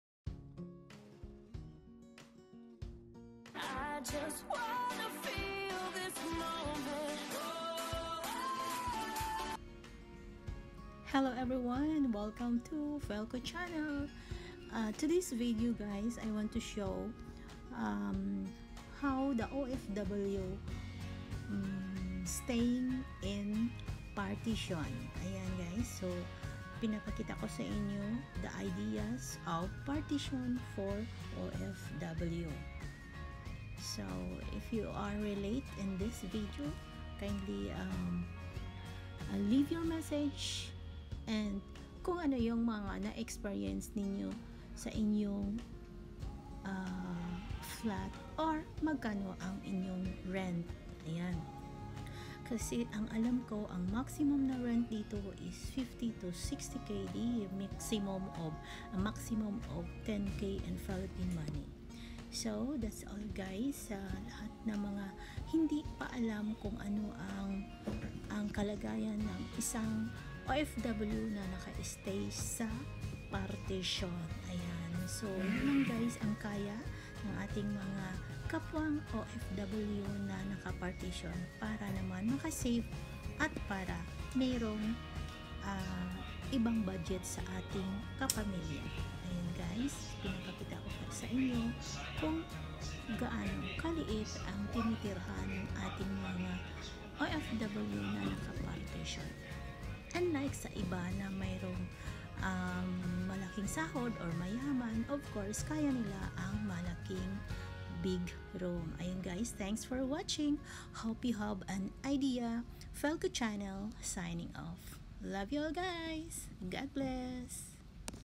MAGKANO ANG PARTITION RENT SA MGA OFW
How Ofw Staying in one Partition room, in order to survive
at makasave for family budget